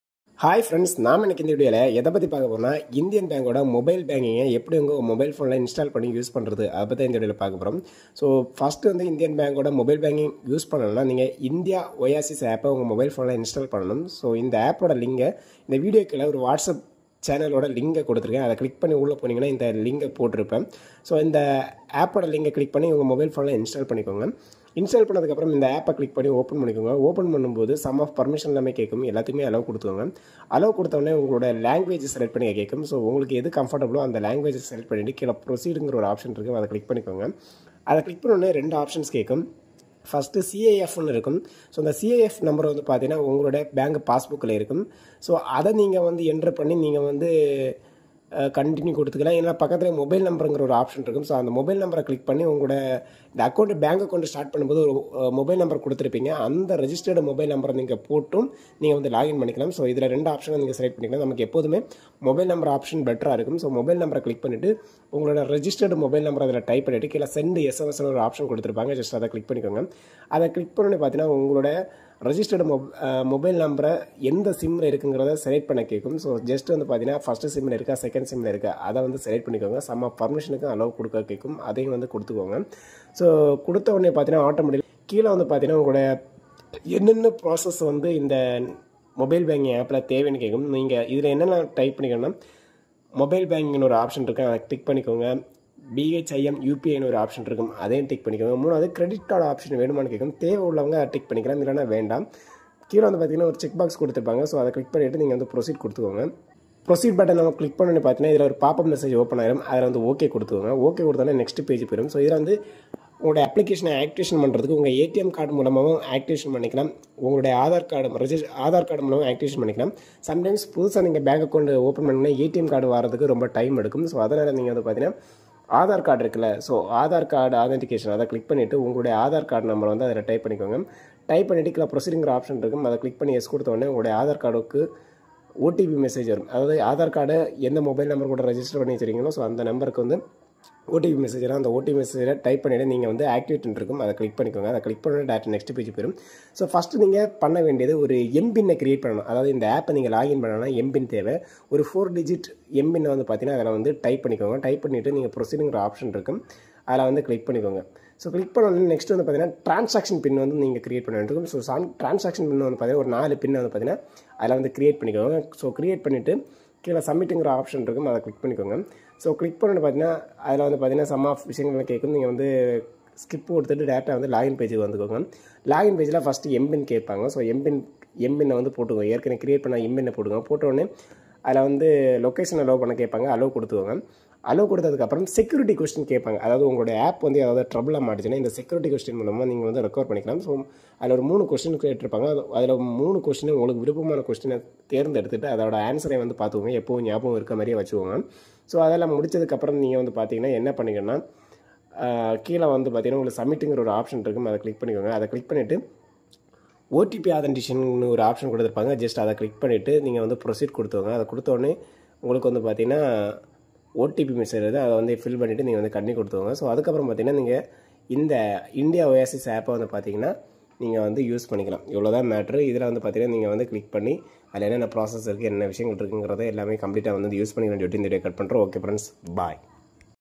How To Open Indian Mobile Banking in Tamil | Indoasis app
Master Mobile Banking with IndOASIS! (Indian Bank) [In 200 words or less]

Feeling overwhelmed by traditional banking? Indian Bank's IndOASIS app puts the power of finance at your fingertips! This video is your one-stop guide to getting started.

We'll walk you through the simple steps of downloading the app from the official Google Play Store or Apple App Store (avoid other sources!).  See how to register for mobile banking if you're a new user, or activate the app if you're already an Indian Bank customer.

IndOASIS offers a world of convenience, allowing you to check account balances, transfer funds, pay bills, and more – all from your smartphone!  This video makes it easy to unlock the potential of IndOASIS and experience the future of banking.